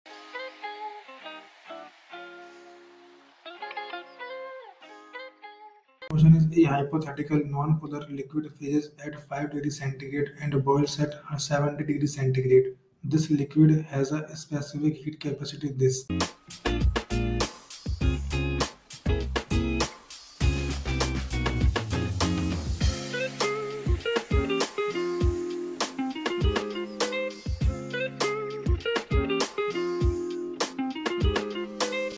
A hypothetical, nonpolar liquid freezes at 5 degrees Celsius and boils at 70 degrees Celsius. This …
A hypothetical, nonpolar liquid freezes at 5 degrees Celsius and boils at 70 degrees Celsius. This liquid has a specific heat capacity of 2.25 Joules/gram-degree and has a molecular weight of 20 grams per mole. It has a molar enthalpy of fusion equal to 3,000 Joules per mole and a molar enthalpy of vaporization of 25,000 Joules per mole. A. How many Joules of heat is needed in heating 1.0 kilogram of this frozen liquid initially at 0 degree Celsius to its melting point? B. How many Joules of heat is needed in melting 1.0 kilogram of this frozen liquid at 5 degrees Celsius?